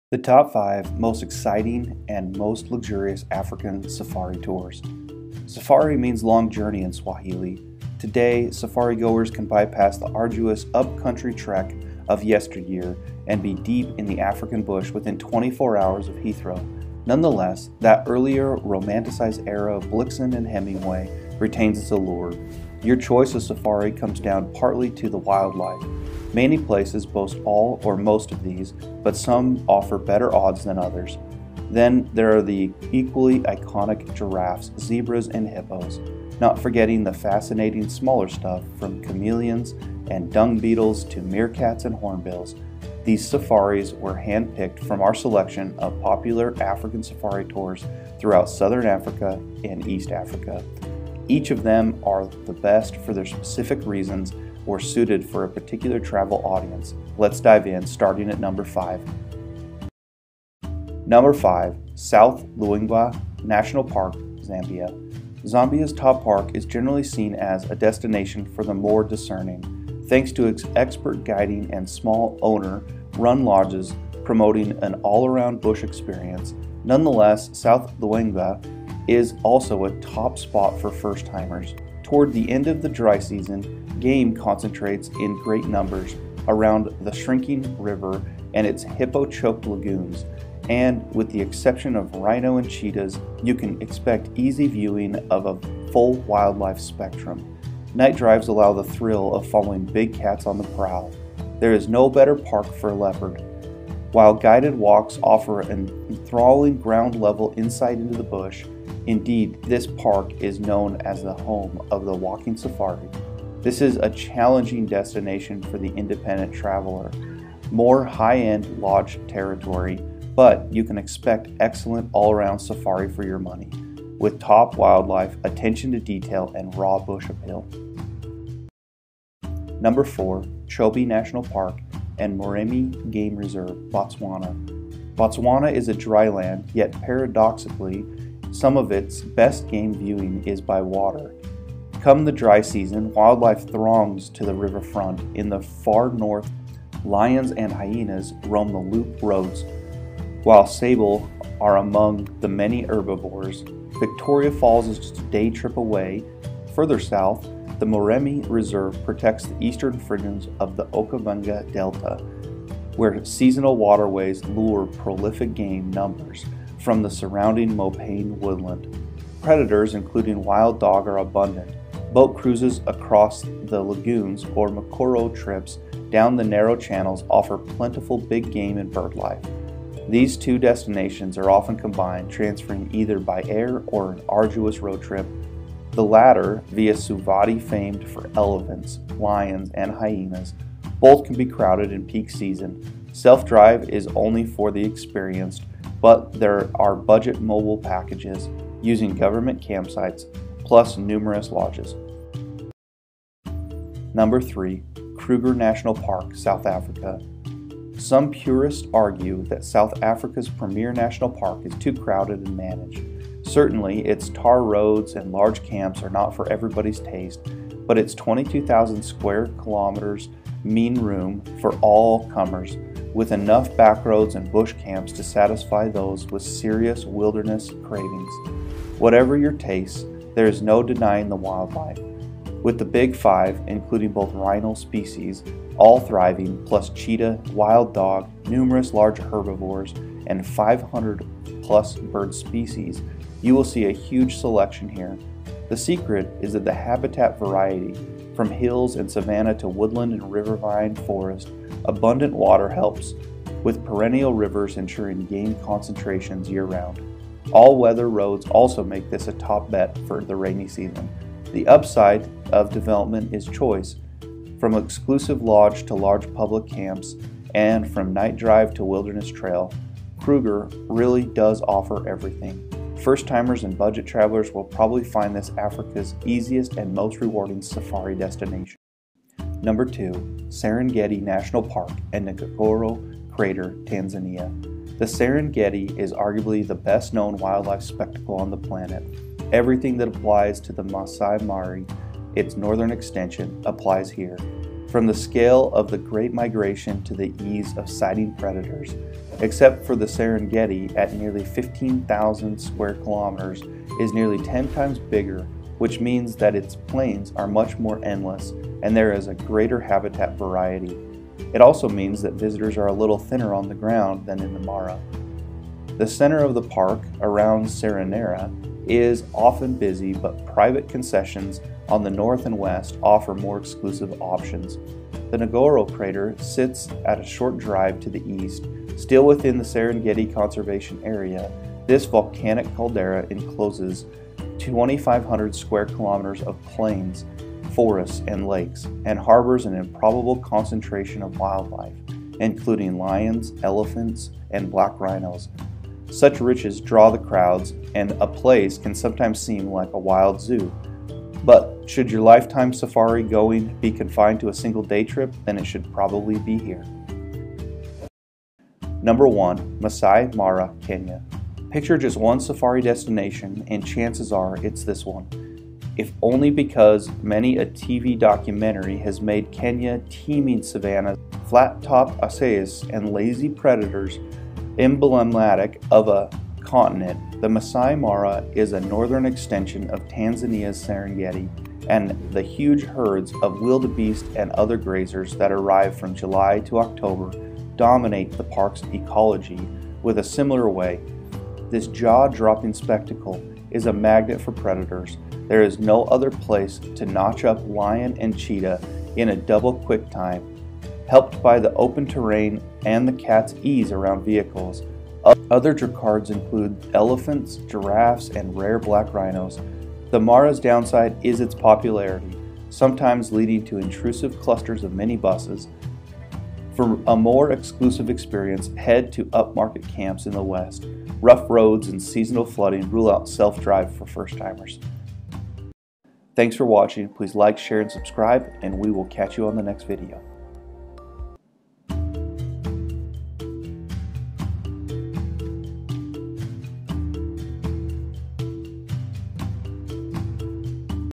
African Safari's You Have to See to Believe | African Safari Tours
Safari means ‘long journey’ in Swahili. Today, safari-goers can bypass the arduous up-country trek of yesteryear and be deep in the African bush within 24 hours of Heathrow. Nonetheless, that earlier, romanticized era of Blixen and Hemingway retains its allure.

Your choice of safari comes down partly to the wildlife. Many places boast all or most of these, but some offer better odds than others. Then there are the equally iconic giraffes, zebras and hippos – not forgetting the fascinating smaller stuff, from chameleons and dung beetles to meerkats and hornbills.  These safaris were hand-picked from our selection of popular African safari tours throughout Southern African and East Africa. Each of them are best for a specific reason or suited to a particular travel audience.

5. South Luangwa National Park, Zambia
4. Chobe National Park & Moremi Game Reserve, Botswana
3. Kruger National Park, South Africa
2. Serengeti National Park and Ngorongoro Crater, Tanzania
1. Masai Mara, Kenya